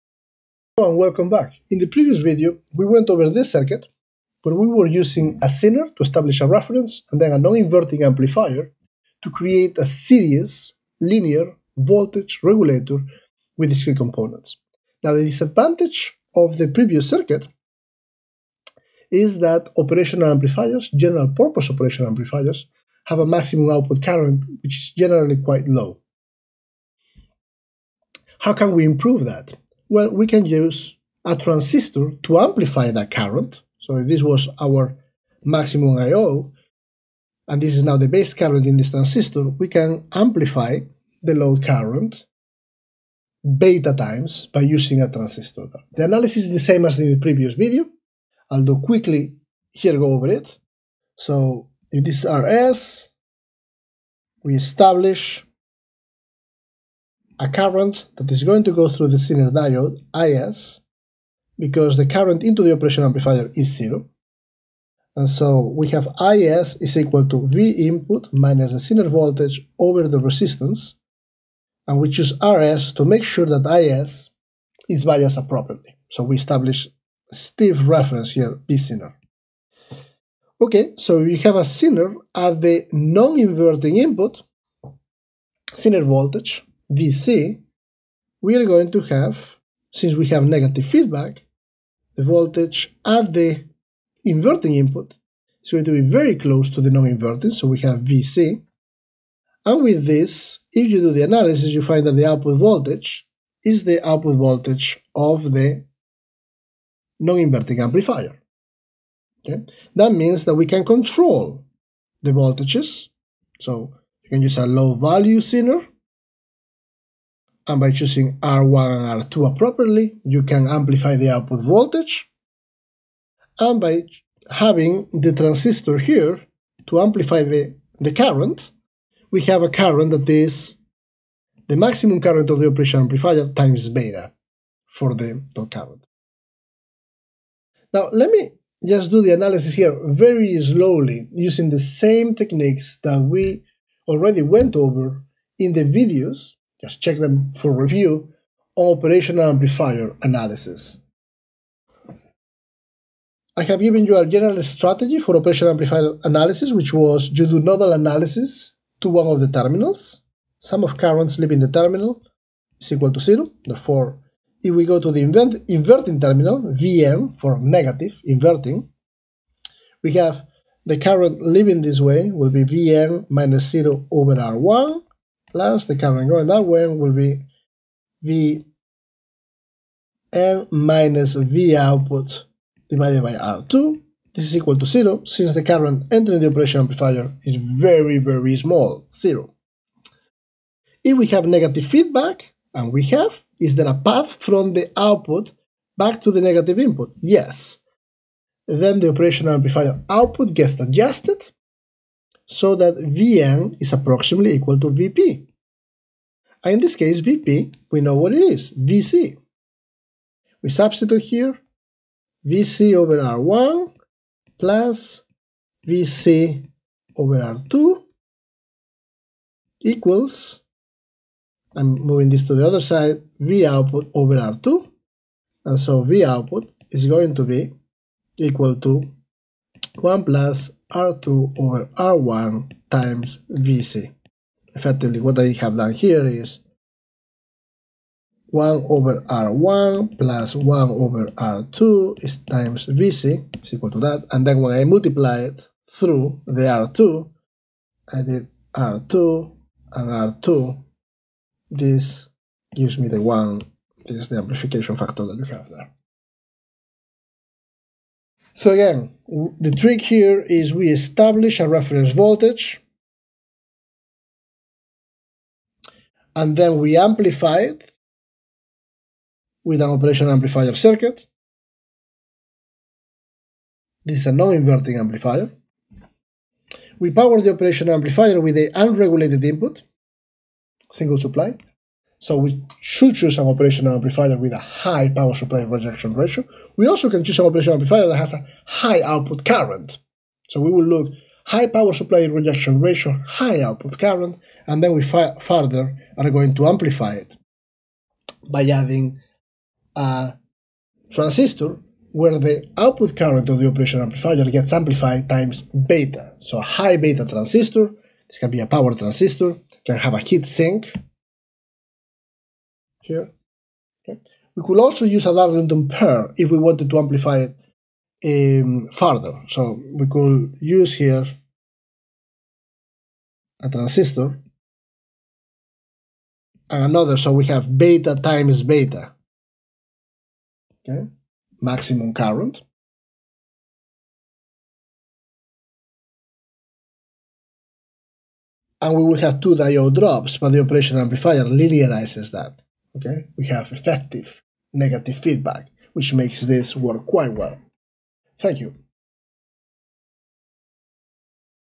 Linear Series Discrete Adjustable Voltage Regulator
This video shows how to analyze and design a linear series voltage regulator with an adjustable output using discrete components including a zener diode, an operational amplifier, and a voltage regulator